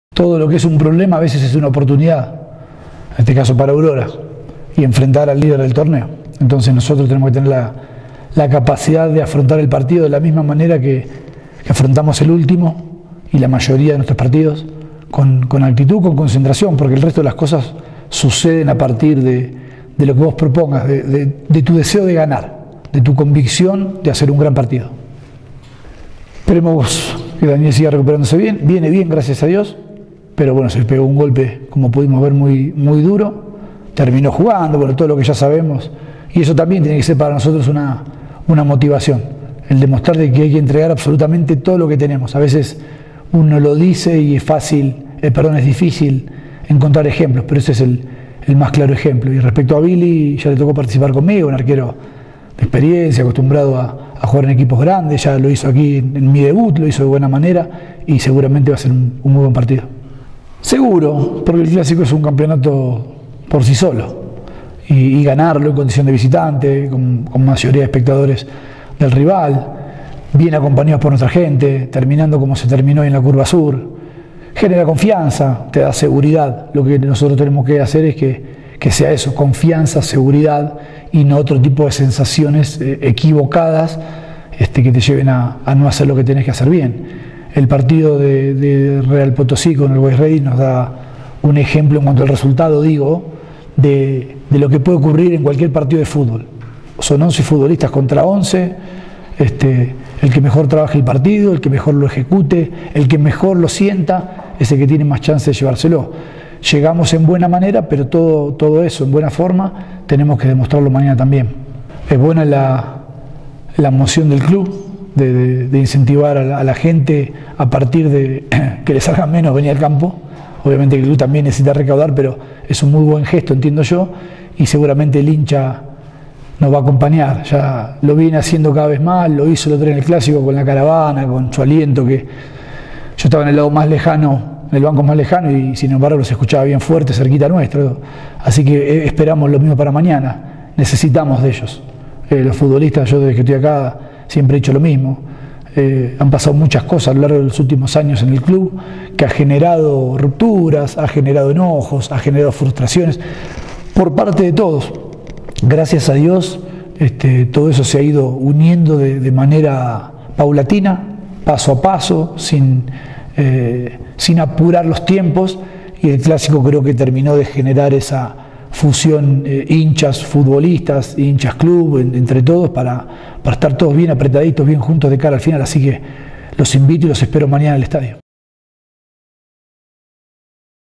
CON EL EQUIPO LISTO
CristianDíaz habló antes del partido de este viernes en el Siles ante Aurora, TheStrongest espera cauto el momento. Más información en CompetenciaBolivia de lunes a viernes a las 13 y 19 hrs.